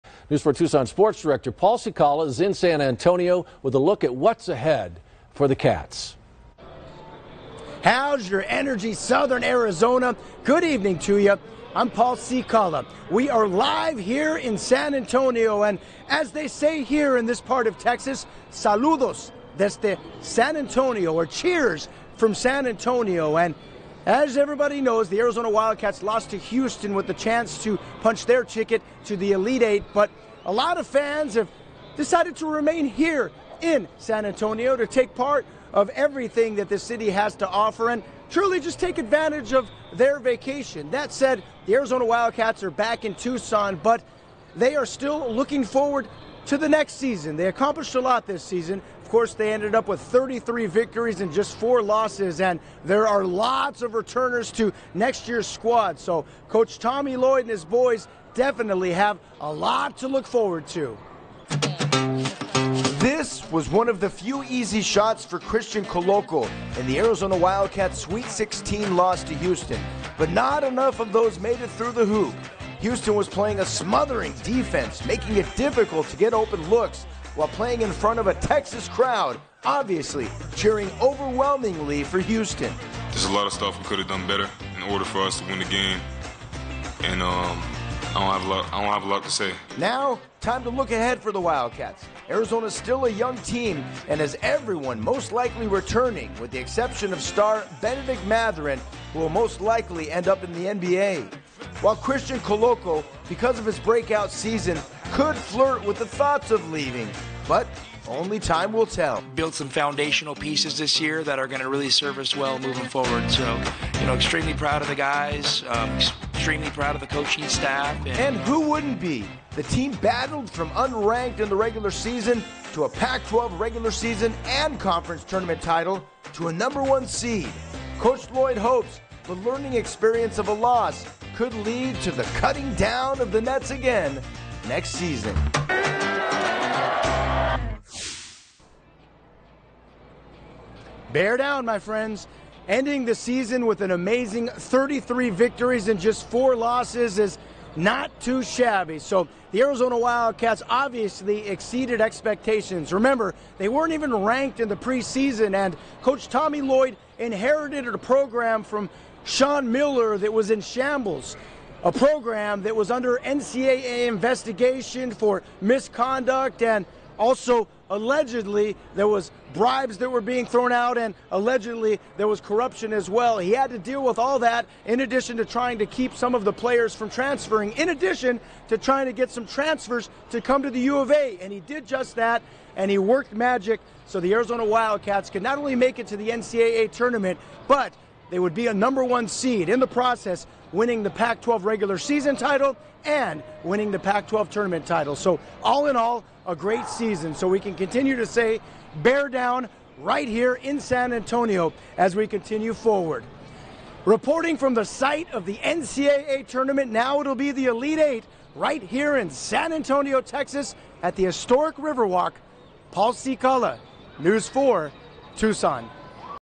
UA hoops returning major talent
News 4 Tucson Paul Cicala reports from San Antonio and talks about the future of the Wildcat men's basketball team (who'll undoubtedly be ranked in the Top Ten -if not higher- beginning next season)